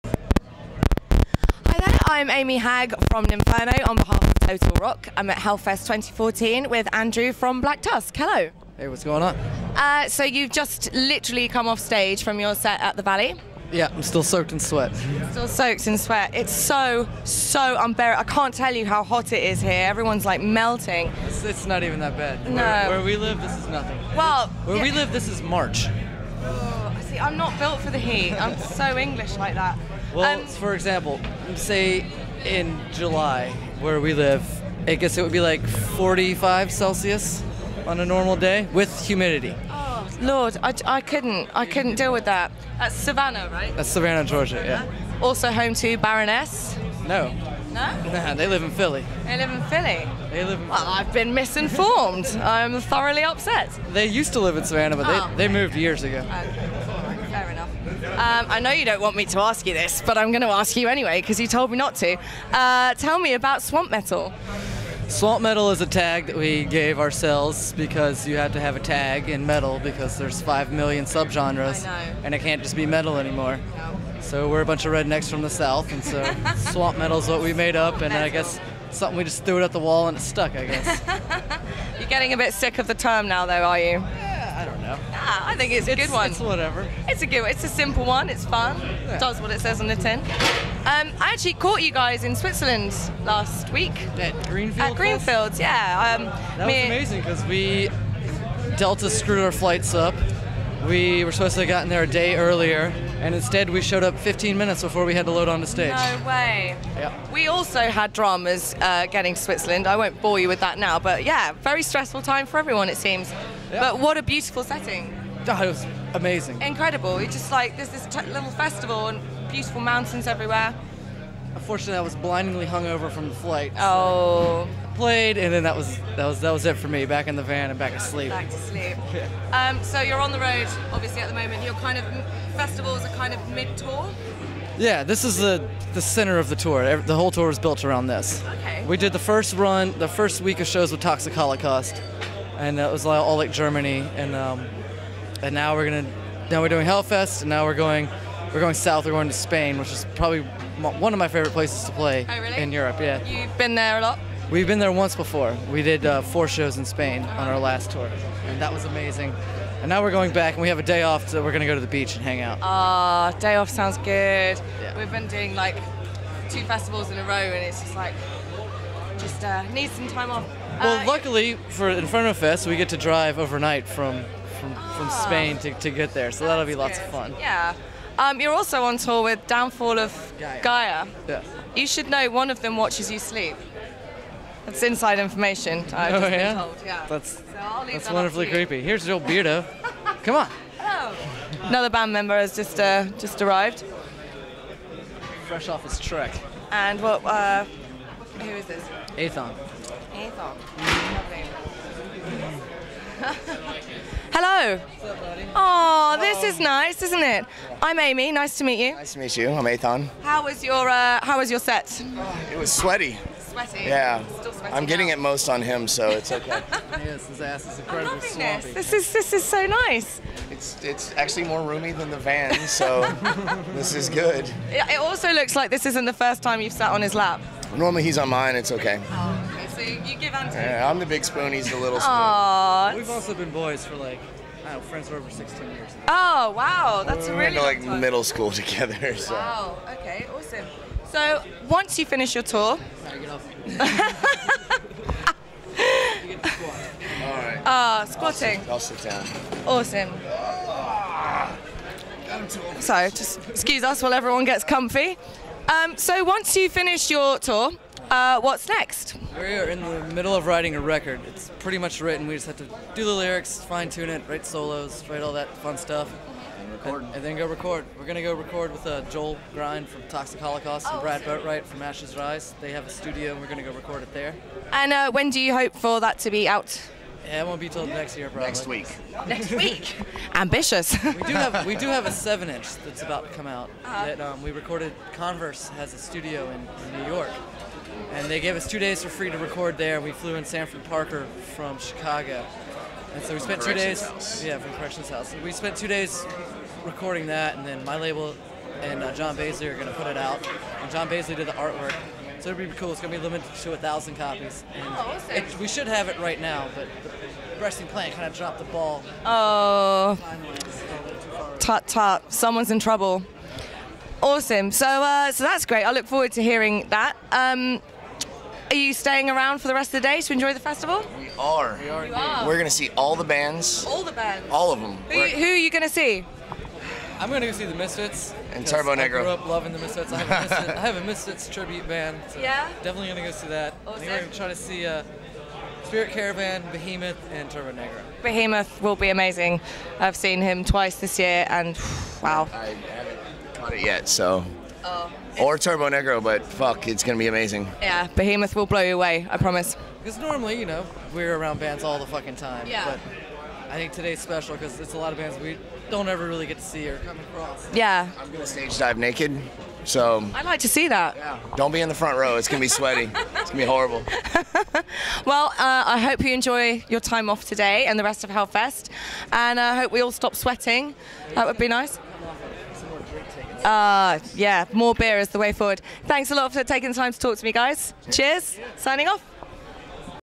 Amy Hag interviews Black Tusk for TotalRock at Hellfest 2014
Amy Hag from Nympherno met Black Tusk's Andrew Fidler (soon joined by Athon...on his lap) at this year's Hellfest in France.